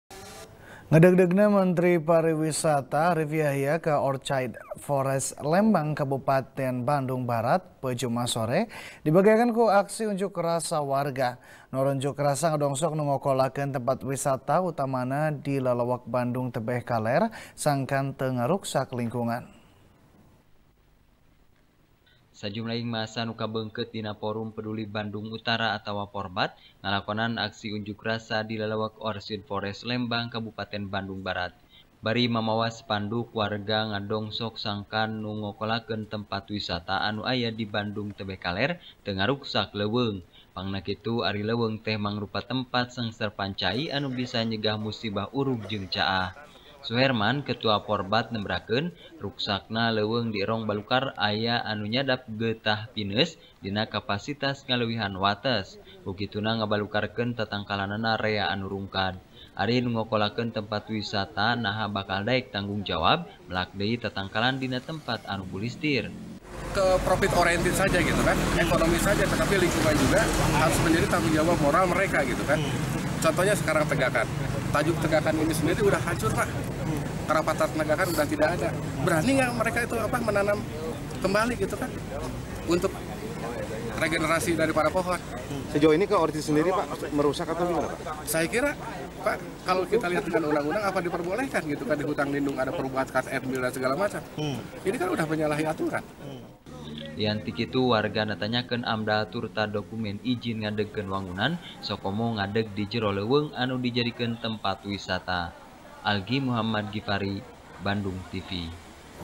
Menpar anu ngadeugdeug ka Lembang dibageakeun demo warga | TP | BANDUNG TV
TP | BANDUNG TV | DERI FIRMAN | Sajumlahing massa anu kabeungkeut dina forum peduli Bandung utara atawa forbat, ngalakonan aksi unjuk rasa, di lelewek orchid forest, Lembang, kabupaten Bandung Barat. Bari mamawa spanduk, warga ngarongsok sangkan nu ngokolakeun wisata anu aya di Bandung tebeh kaler, teu ngaruksak leuweung. Pangna kitu ari leuweung teh mangrupa tempat sengserepan cai, anu bisa nyegah musibah  urug jeung caah.